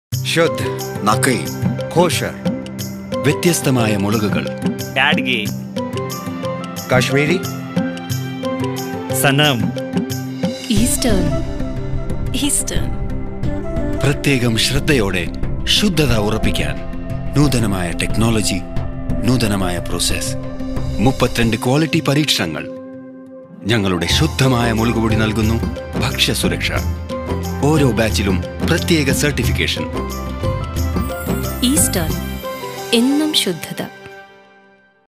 Eastern Chilli Powder – Safe and Quality Tested
We at Eastern are dedicated to bringing only the highest quality products made from the highest quality ingredients to store shelves and your very homes. This is why the ingredients we use undergo rigorous testing before making it into our products.

Eastern chilli powder is made using carefully selected and hygienically picked chillies that undergo 32 quality tests. This ensures that we filter out all the produce that is unfit for consumption and proceed to make our chilli powder only with the very best chillies we could obtain. The strict regulations we follow means that each batch of Eastern products adheres to quality standards like GMP, HACCP and ISO. It is only with these quality certifications that each of our products can sell across the international markets we cater to. Thus, we ensure that our customers, no matter where in the globe they are, obtain only the very best ingredients to use in their kitchens. Eastern believes in delivering products that are 100 % safe for consumption.

We’d love to tell you more in detail about how we ensure product quality.